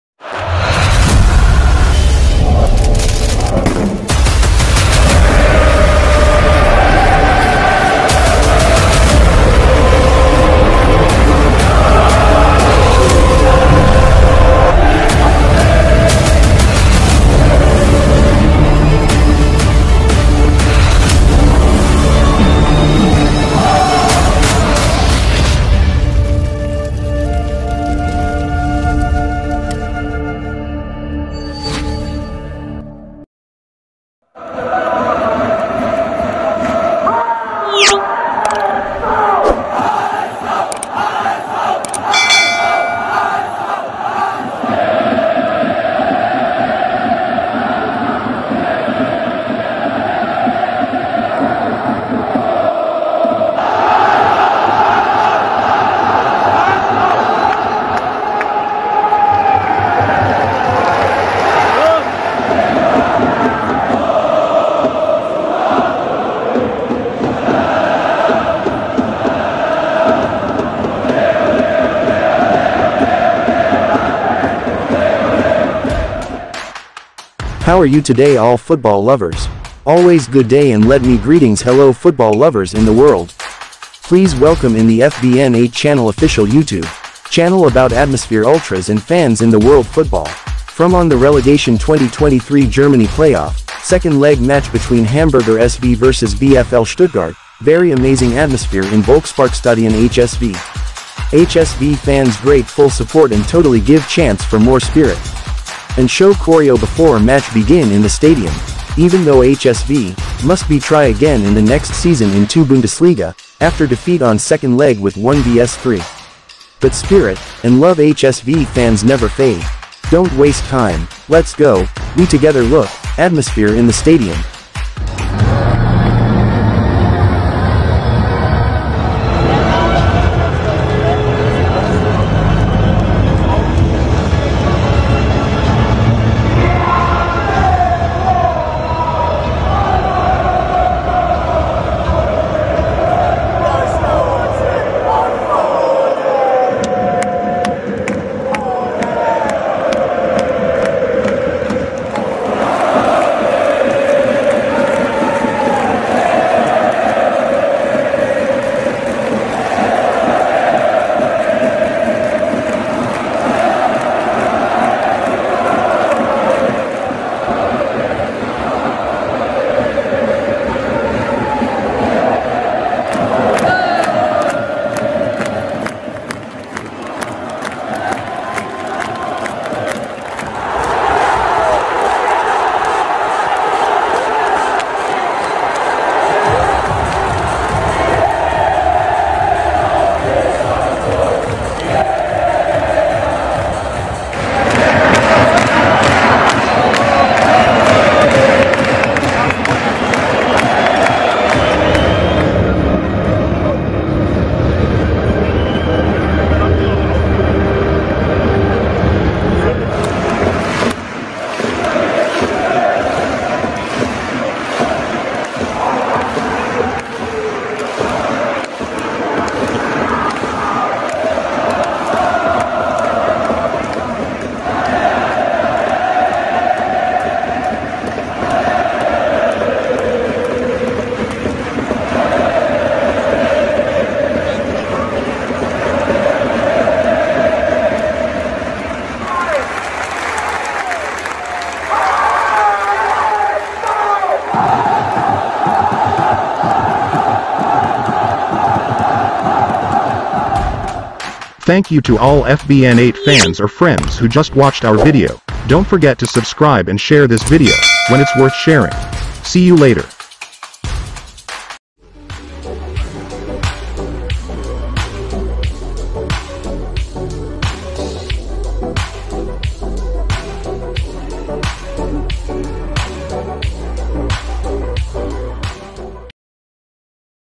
🔵⚪⚫ HSV FANS SHOW CHOREO IN PLAYOFF RELEGATION 2023 • Second Leg • Hamburger SV vs VfL Stuttgart
▶️ FBN8 YouTube Channel

We will immediately respond to remove this video or we will find another solution together.

Match : Hamburger SV vs VfL Stuttgart
League : Relegation 2023 🇩🇪 Second Leg
Score : 1-3 (06/06/2023)
Stadium : Volksparkstadion.

About Video : HSV Fans full support with choreo on the second leg Relegation 2023 Playoff.

Get the new Video Everyday about Fans Atmosphere in a World.